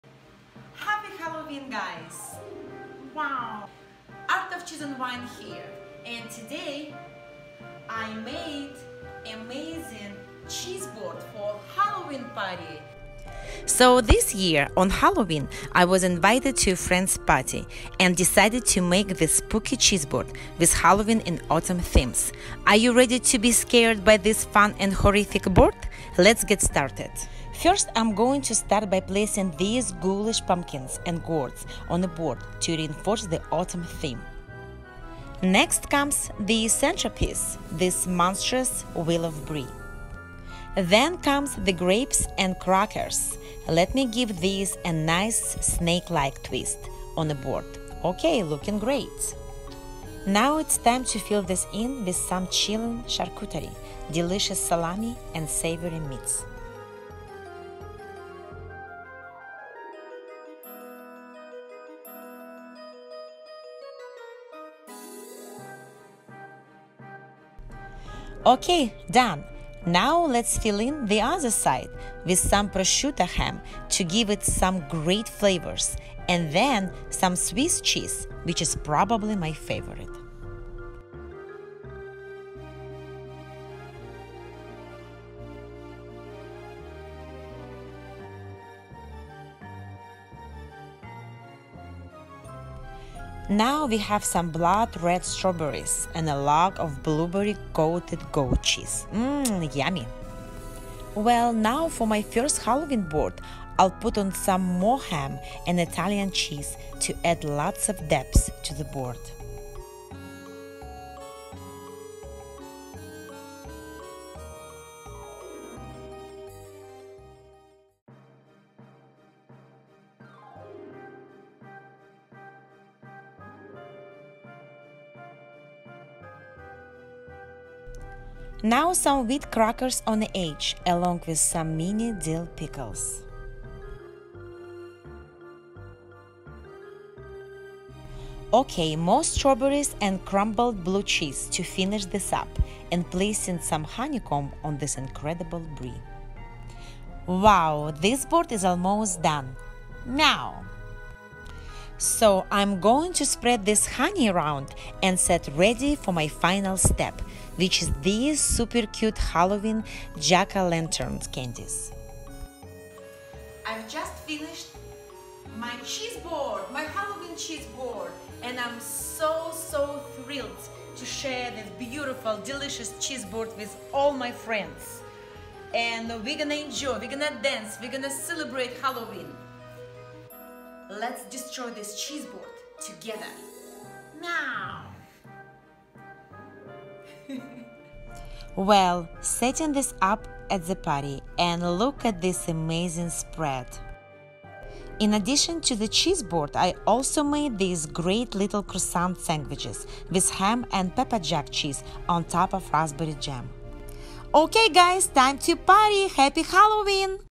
My First Halloween Cheese Board 👻
Hi Guys, Halloween might be over but I’ve finally put together my Halloween Cheeseboard video for all of you and I’m so happy to share this tonight!
I had the chance to attend a grown up Halloween party with no kids for the first time ever and was so excited that I decided to make a Halloween themed board. 🎃🧀🍇
And of course, I needed a costume so I went as a beautiful black cat! Meow! 😻